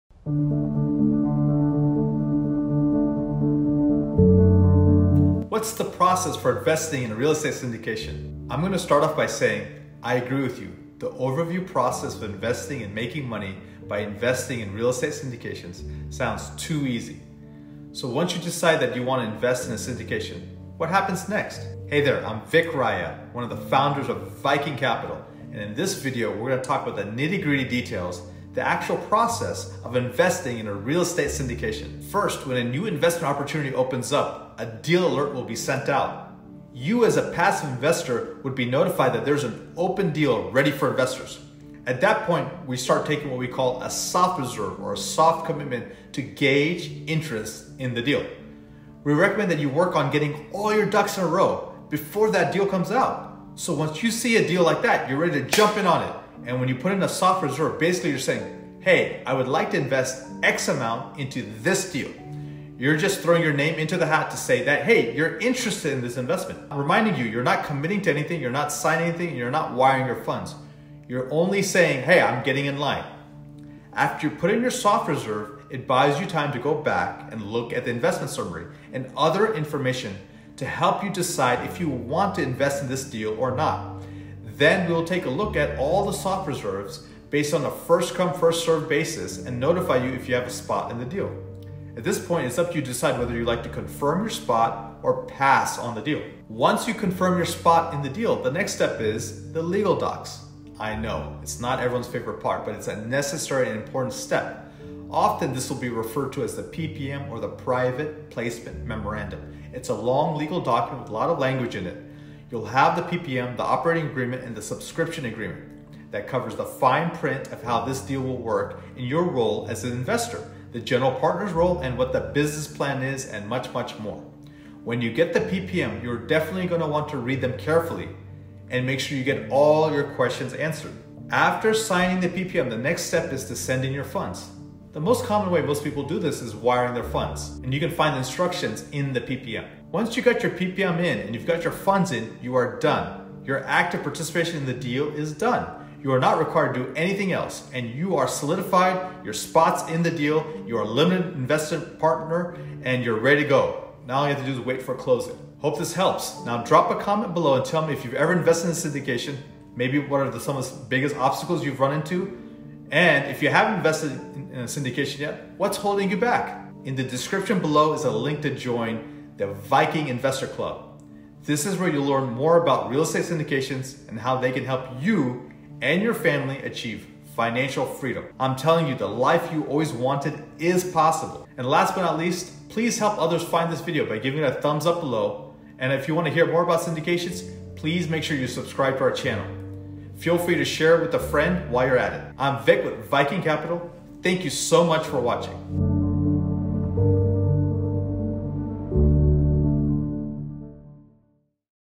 The Process of Investing in a Real Estate Syndication
So once you decide that you want to invest in a real estate syndication, what happens next?

In this video, we’re talking about the nitty-gritty details, the actual process of investing in a real estate syndication. Deal analysis, soft reserves, firm commitments, and wiring funds are all common terms used after deciding to invest in a real estate syndication. Watch this video if you want to know exactly what to expect when you decide to invest in a deal!

To learn more about investment opportunities, join the Viking Investors Club